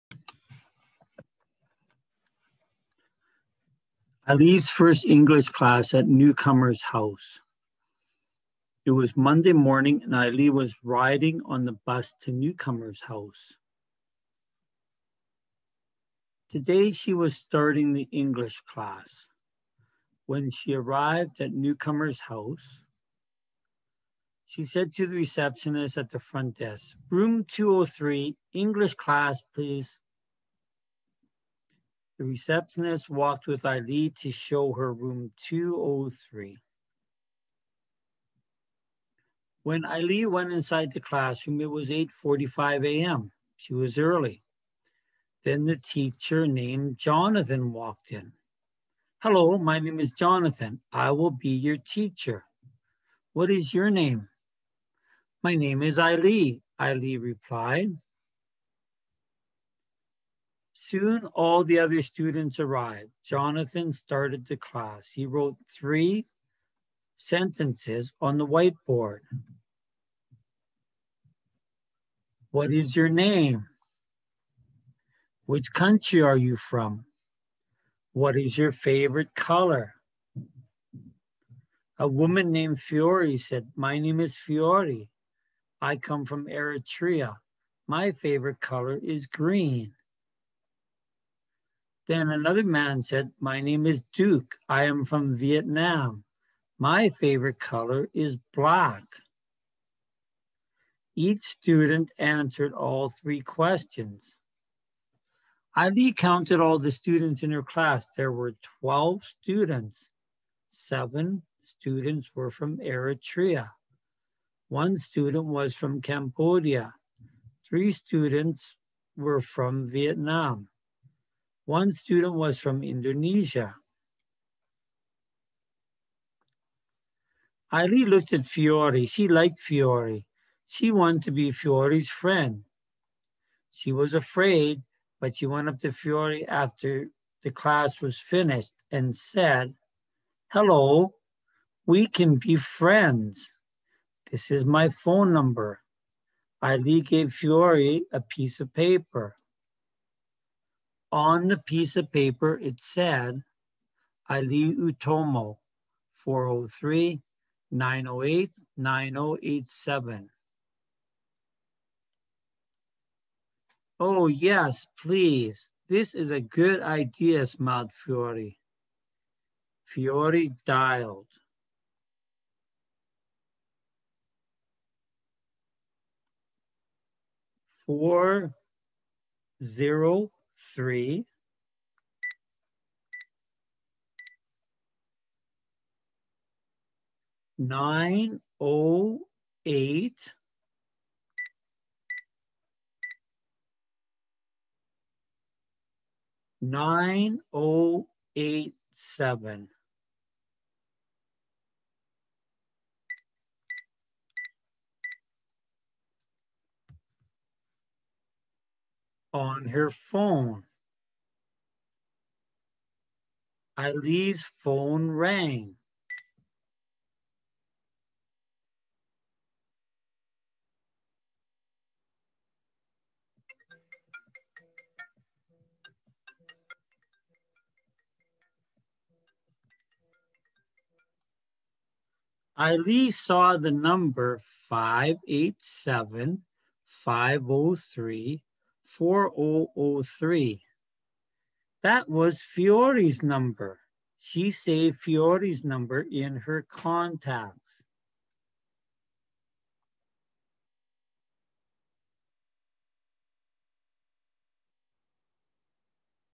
English Conversation [English] - 'Ay Lee's First English Class' (Timothy Navratil) ,Mar 24, 2021
Reading of 'Ay Lee's First English Class' by Timothy Navratil